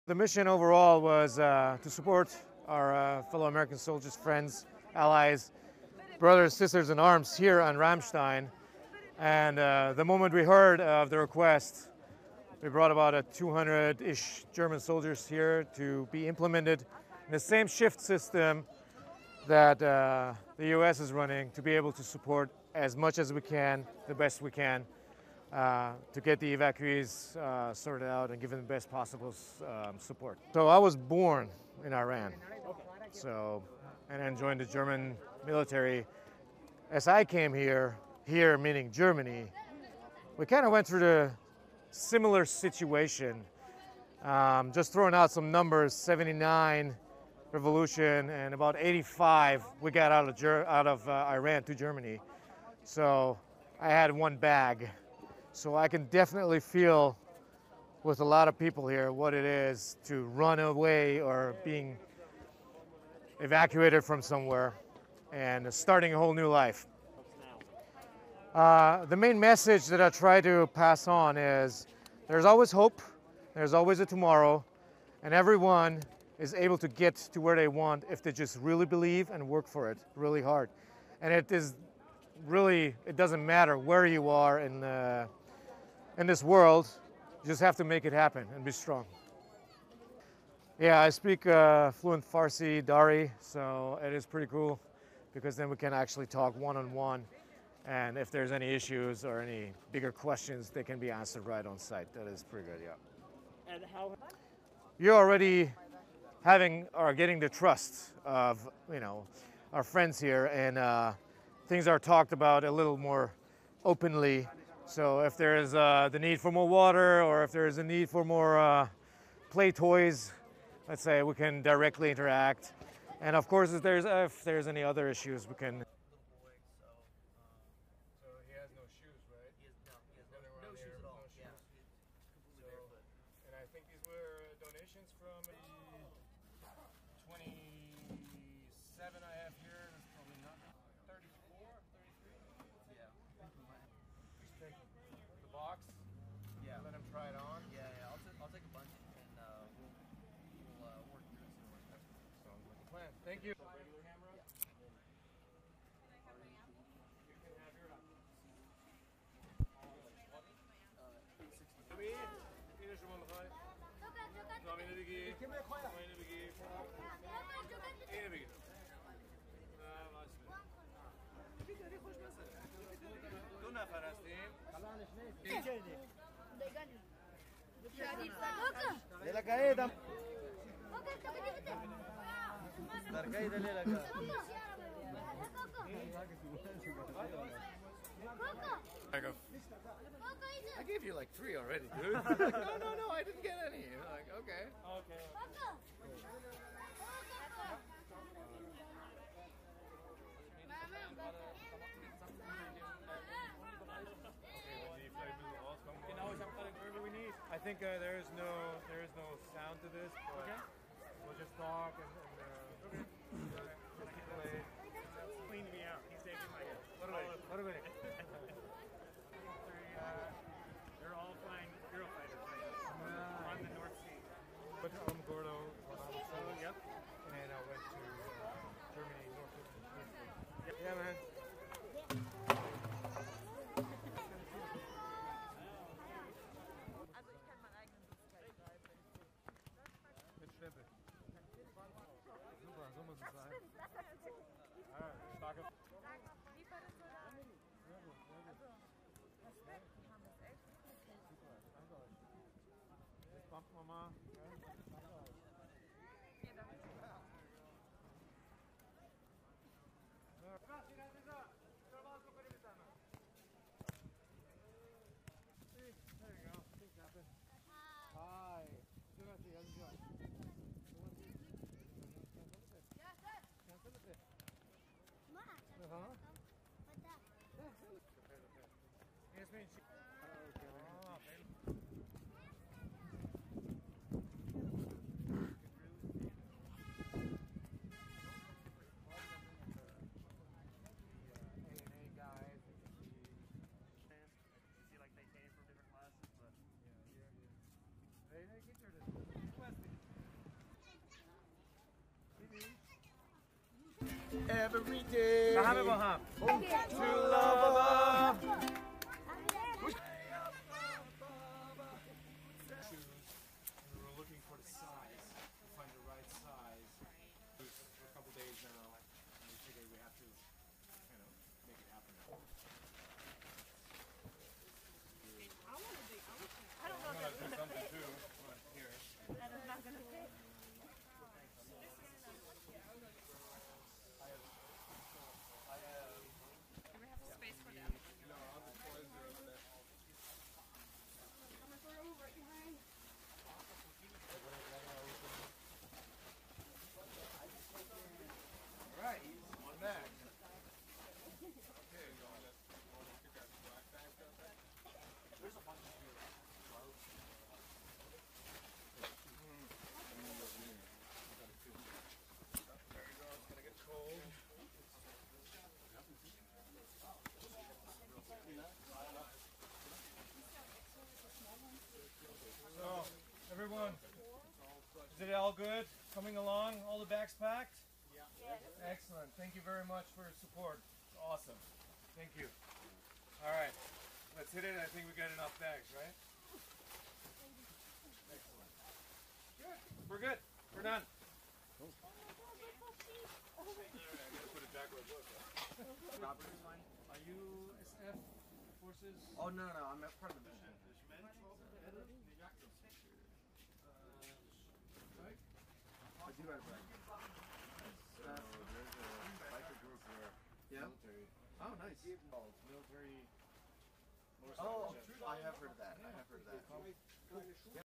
B-Roll Lt. Col Samadi Assists Evacuees @ RAMSTEIN AIR BASE, RP, GERMANY
B-Roll Lt. Col Samadi Assists Evacuees
RAMSTEIN AIR BASE, RP, GERMANY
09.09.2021

B-Roll Package - Lt. Col. Nadar Samadi is helping Afghan Evacuees in a way that few can. He speaks English, German and Farsi, allowing him to communicate through multiple groups of people. Col Samadi also draws on his own experiences, having left Iran at age 7 as an evacuee.

Film Credits: Video by Staff Sgt. Samuel O'Brien
86th Airlift Wing/Public Affairs